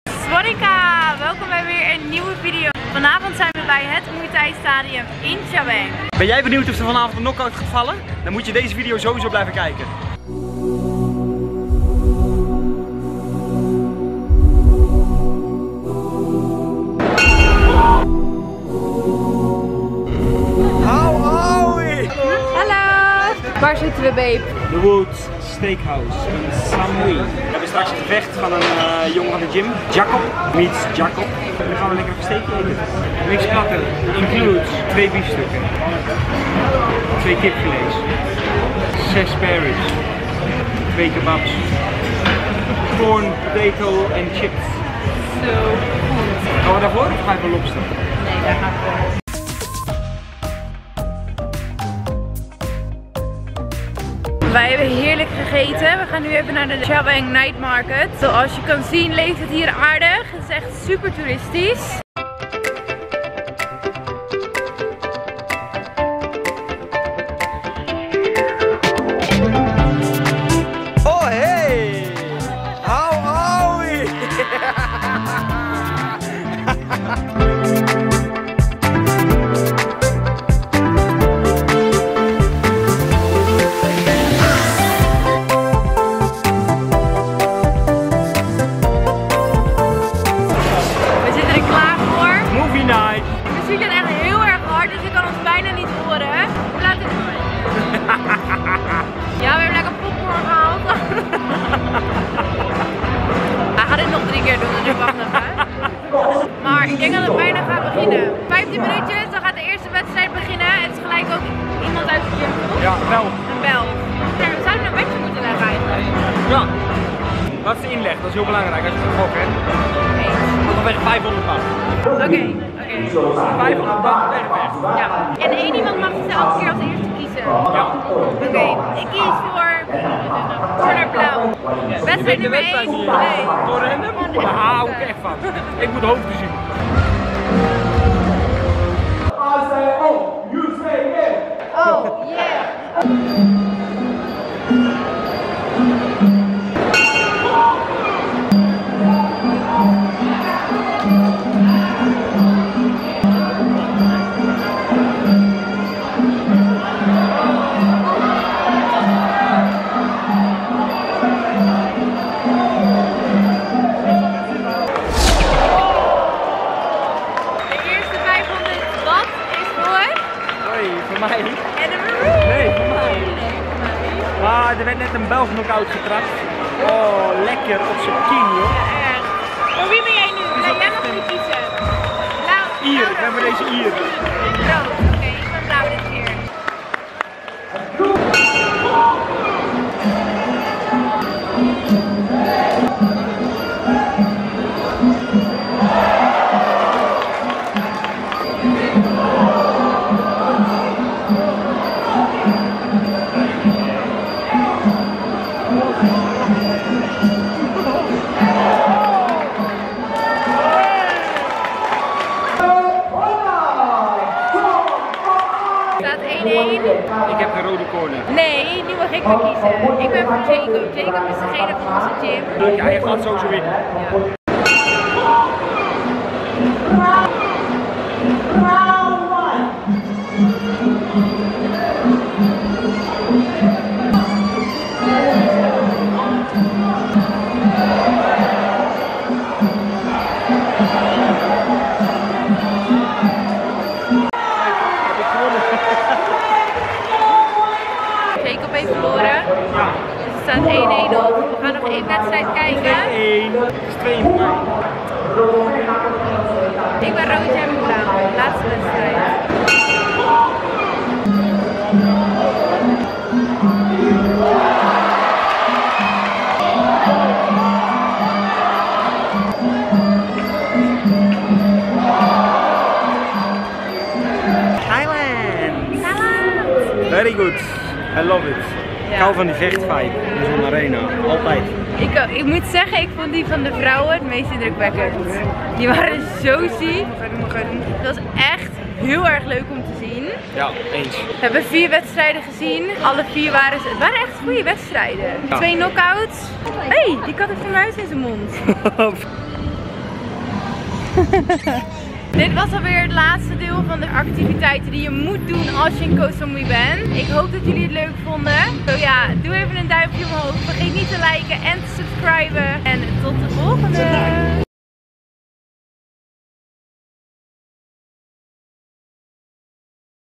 FIGHT NIGHT in Chaweng Stadium | Will There Be A Knock Out?
In deze video nemen Jaap en ik je mee naar het Chaweng Stadion op Koh Samui. Tijdens deze sensationele showdown werd een verwoestende knockout uitgedeeld, waardoor het publiek versteld stond. Het gevecht was een explosieve mix van adrenaline, actie en pure kracht. Deze gevechten mag je absoluut niet missen!